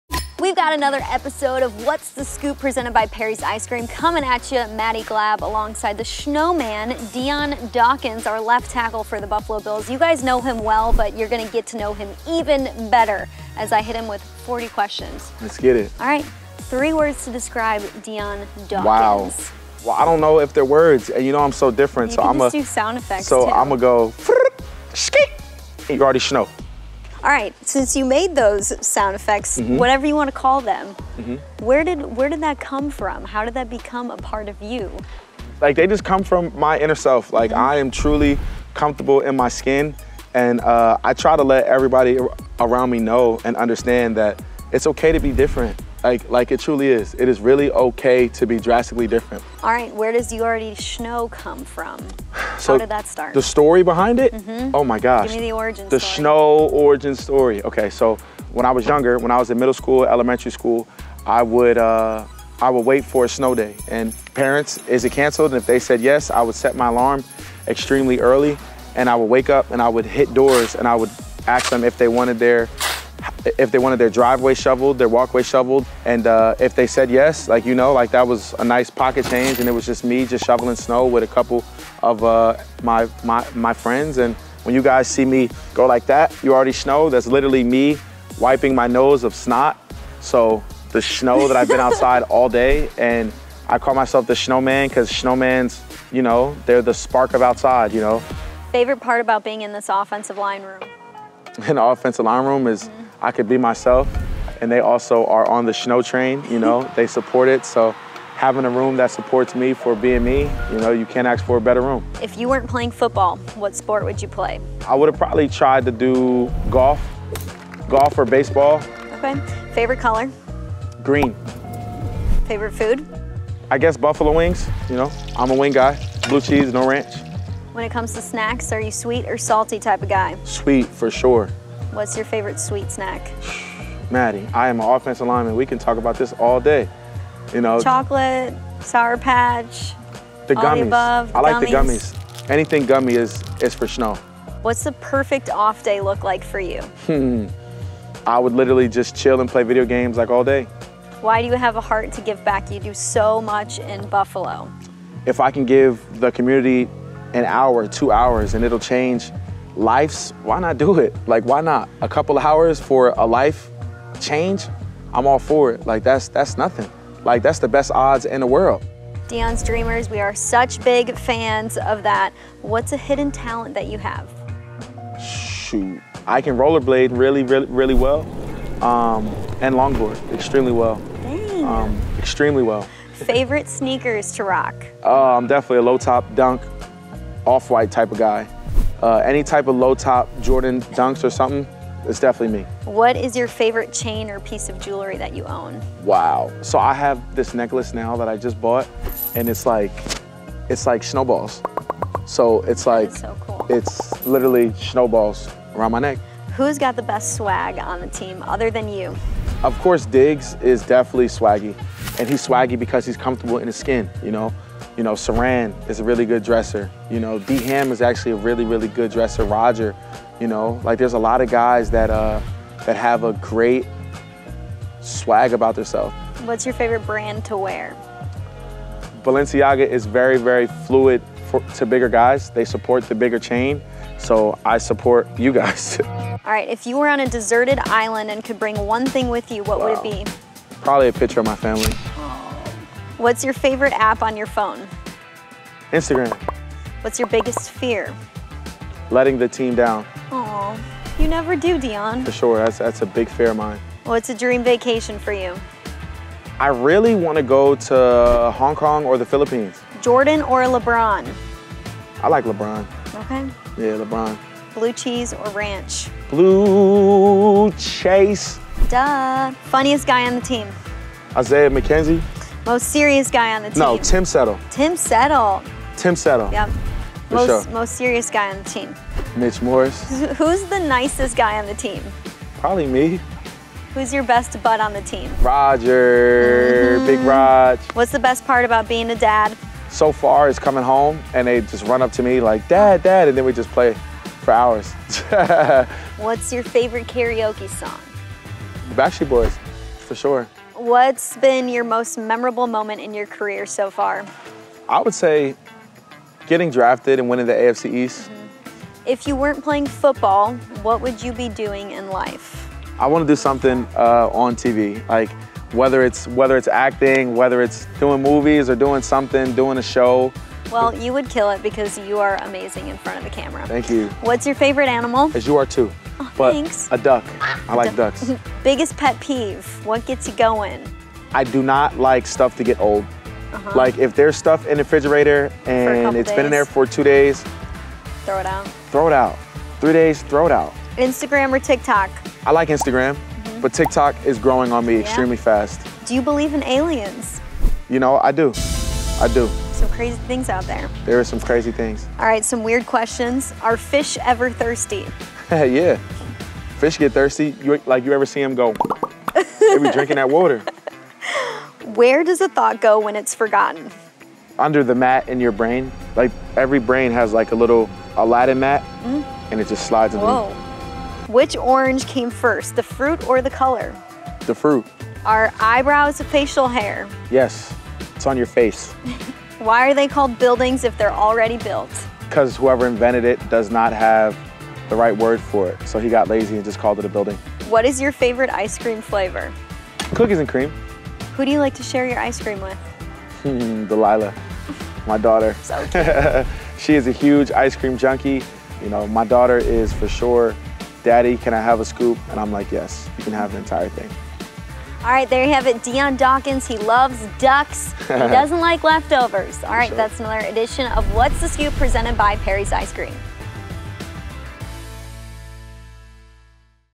40 Questions With Dion Dawkins! | The Origin Of "Shnow", Hidden Talents & Aliens | Buffalo Bills
Watch Bills Left Tackle Dion Dawkins answer 40 questions about himself, including the best dressers on the team, favorite jewelry, family life, dream vacations, and more.

What's The Scoop is presented by Perry's Ice Cream.